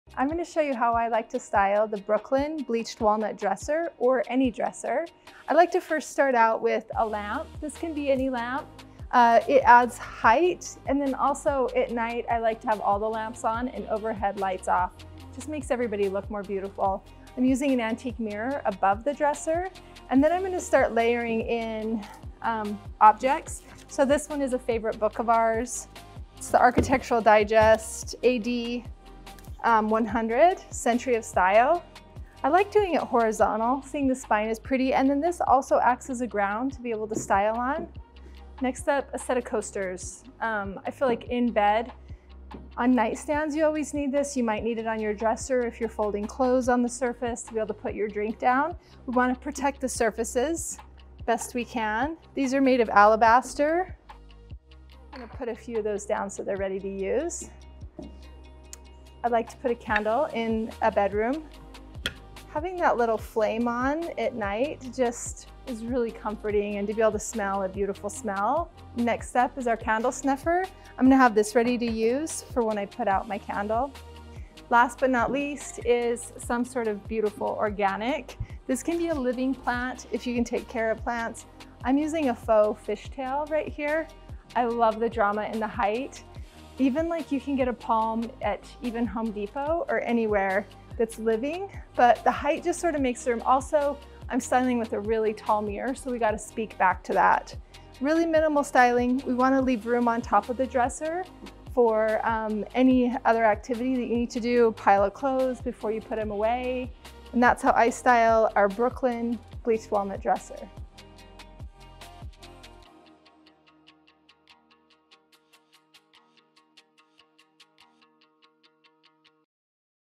Brooklyn Dresser Styling | Alice Lane Home Collection
Let's style our Brooklyn Dresser. First, start with a mirror hung on the wall. It creates a beautiful base and reflects the objects we style. Next, add a lamp like our Beverly Crystal Lamp, it easily lends height and atmosphere. Then, layer in objects like your favorite book. Incorporate a coaster set, this protects the surface and gives a place for late-night drinks to rest. Our Alabaster Coaster Set is perfect for this. Next is adding a signature scent, helping to set a relaxing tone. Pair our Clawfoot Dish and Candle Snuffer for easy candle care and elegance. Finally, bring in something organic, either faux or living. Select a large piece like a Fishtail Palm for height and drama. This creates our favorite way to style bedroom dressers.